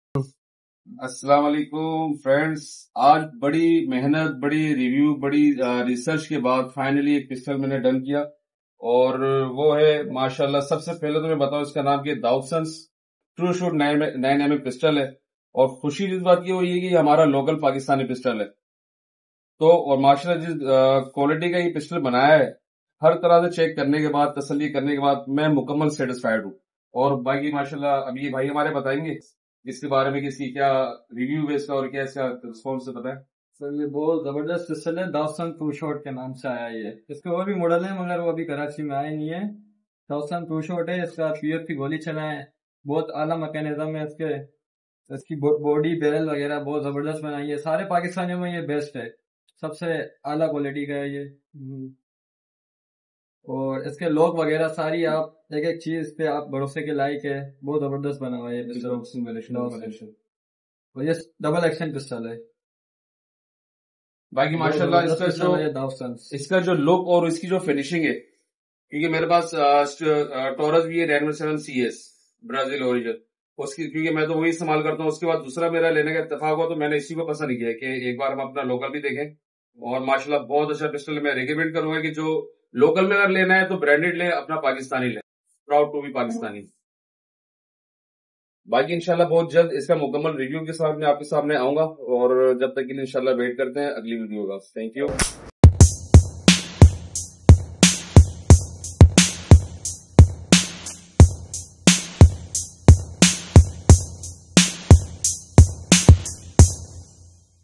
DSA TRUSHOT 9MM PISTOL
daudsons armouryMADE IN PAKISTAN - SINCE 1953 · DAUDSONS ARMOURY (PVT.) LTD. · PESHAWAR - PAKISTAN

DSA TRUSHOT™ 9mm is the first DAUDSONS Handgun. It has become a performance benchmark in Pakistan's highly competitive handgun market. The Trushot™ is reliable, compact and affordable. Salient features include an Alloy frame machined from solid Aerospace grade Aluminum, Forged alloy steel slide and fiber optic sights. The Trushot™ also comes standard with DSA Scratchless™ coatings.